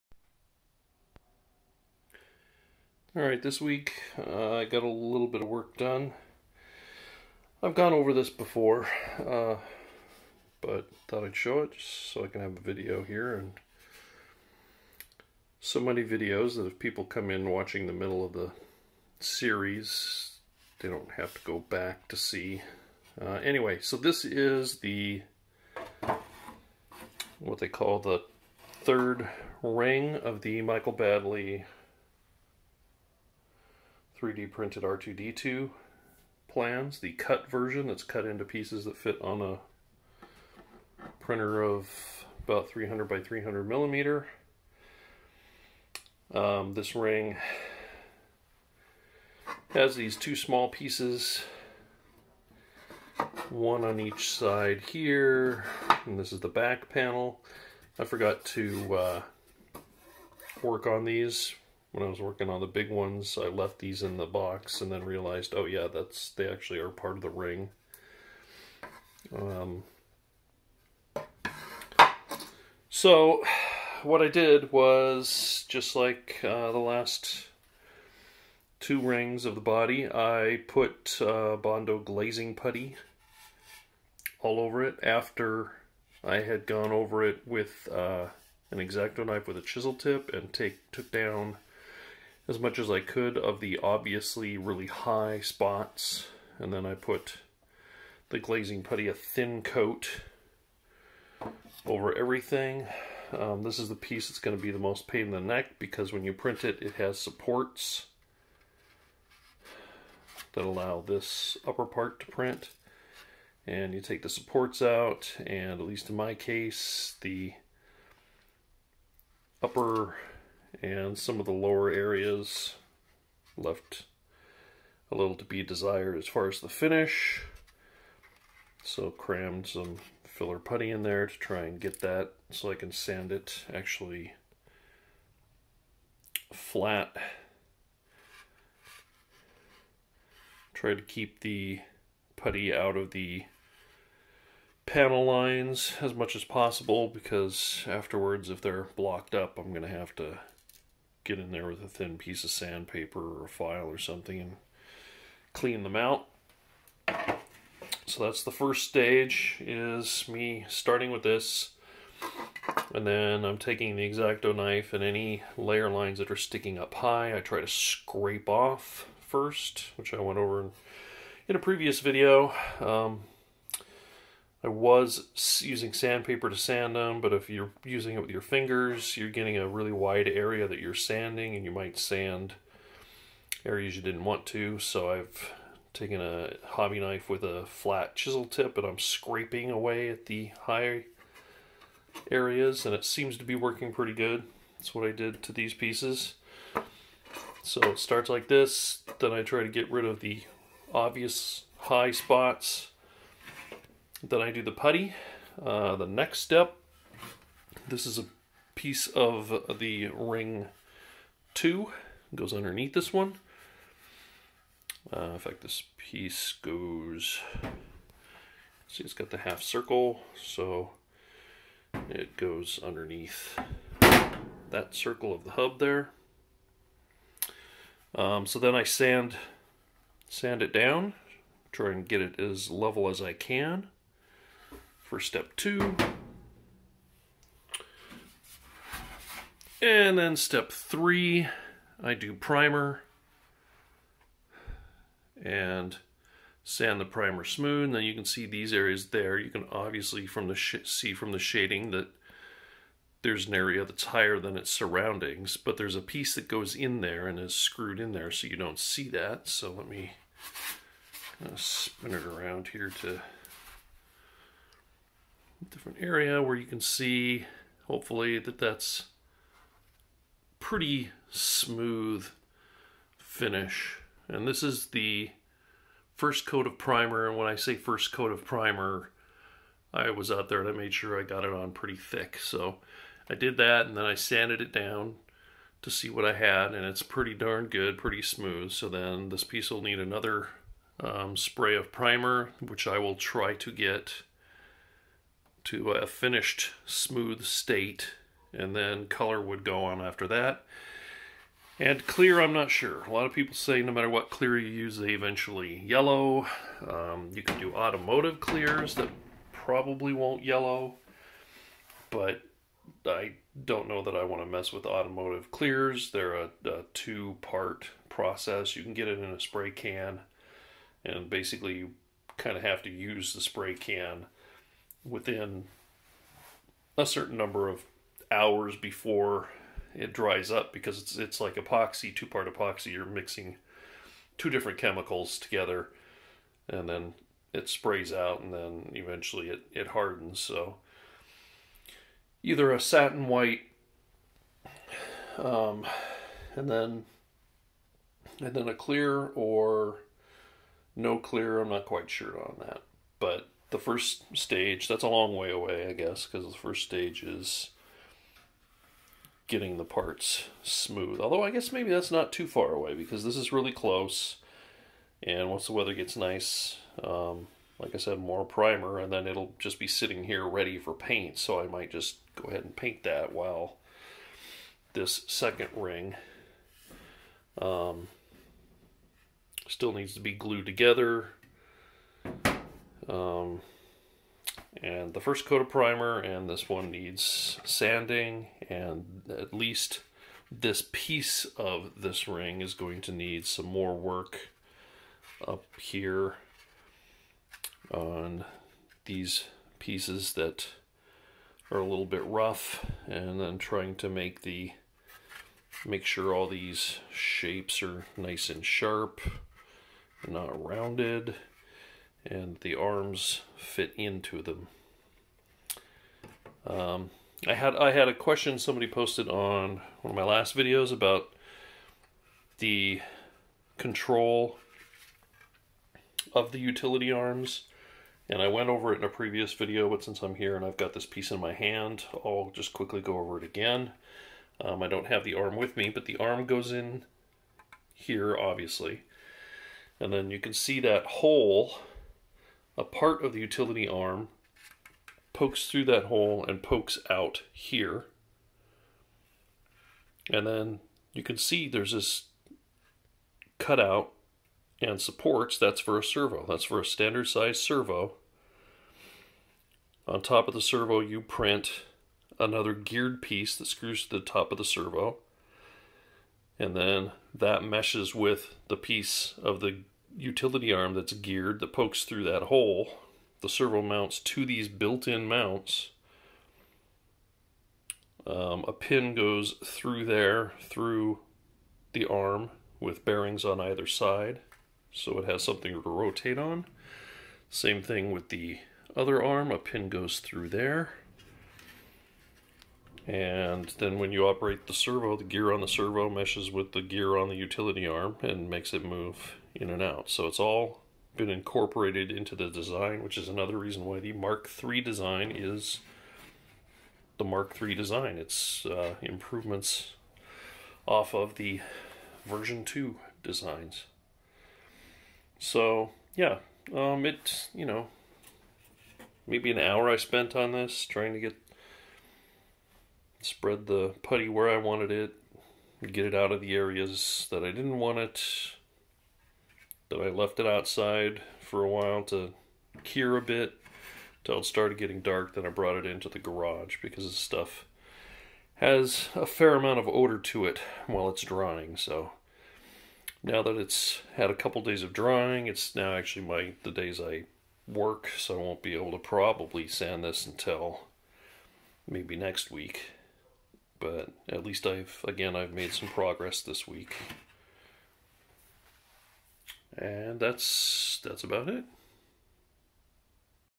3D Printed R2D2 - Part 38
I've showed this bit before, so nothing new.  I applied some filler to the pieces of the 3rd (top) ring of the body.  I did forget that there are 2 smaller pieces that I have printed but didn't prep by sanding/scraping the high spots down.